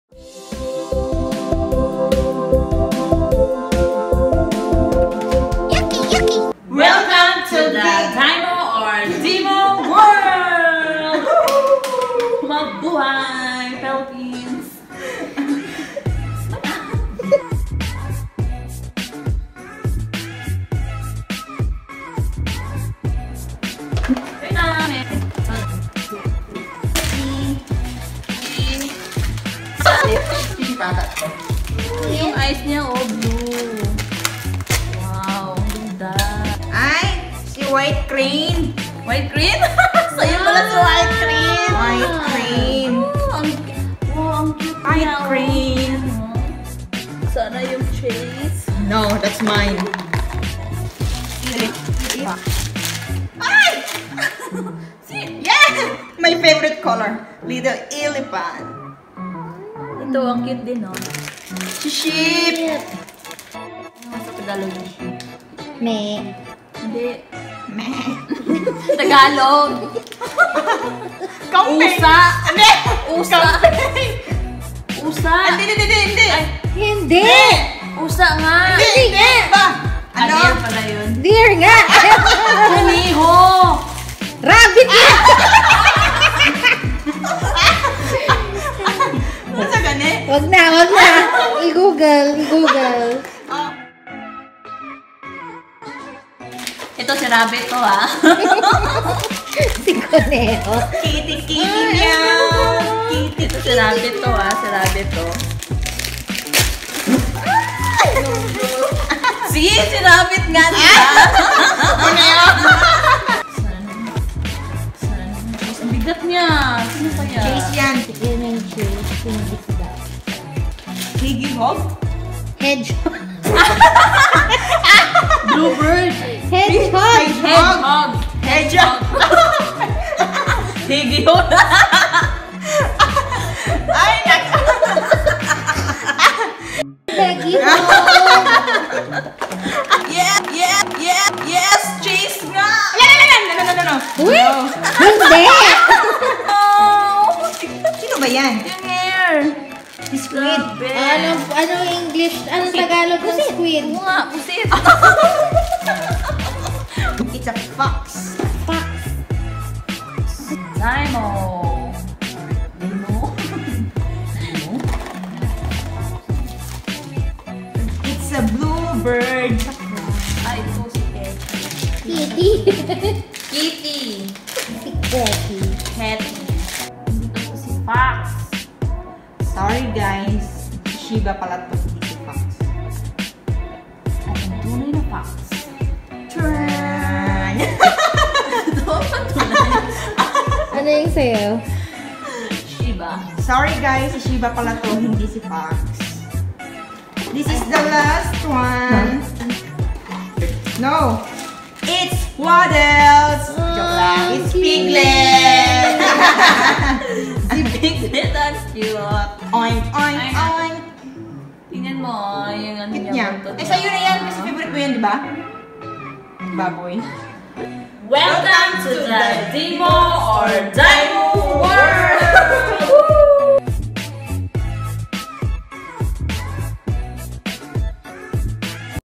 GEEK PH UNBOXING VIDEO: POP MART : DIMOO
(click on HD)

Our unboxing video presented by our very own Geek Girls, Rizza and Melody for the unboxing of Pop Mart's latest offer: Dimoo!

Hope you will have as much watching as we had unboxing!!

Geek PH Flagship Store - 2/F V-mall, Greenhills Shopping Center, San Juan City, Metro Manila, Philippines